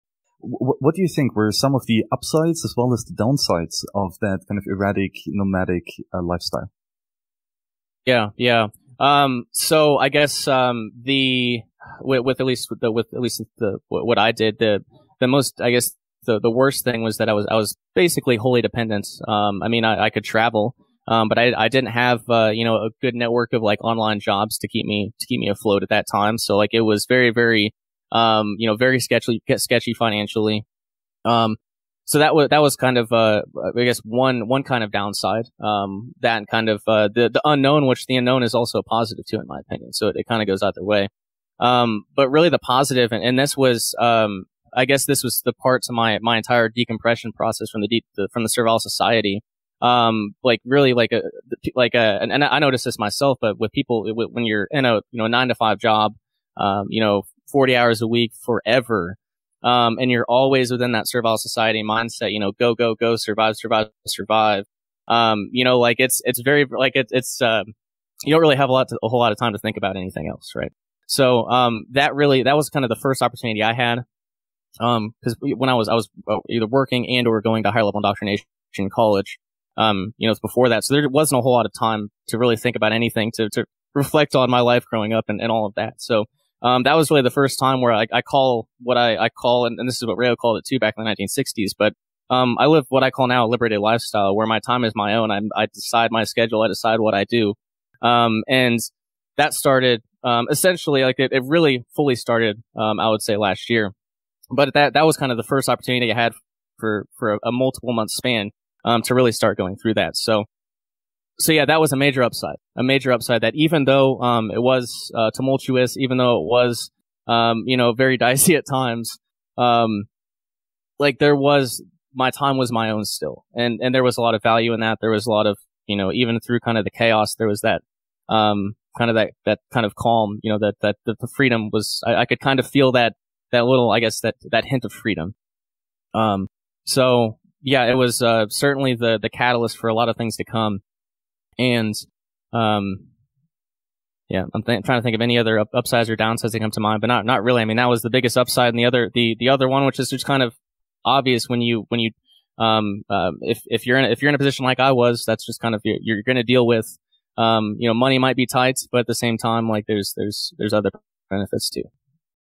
11.4 The Ups and Downs of a Nomad || Shane Radliff
Shane explains that a potential downside for a Nomad is to rely on the income of physical jobs, and how digital entrepreneurship alleviates that problem. The unknown and uncertainty of where to wake up tomorrow is challenging, but empowering. Yet being out of the 9 to 5 rat race gives ample time to contemplate and reflect on the importance of a free life.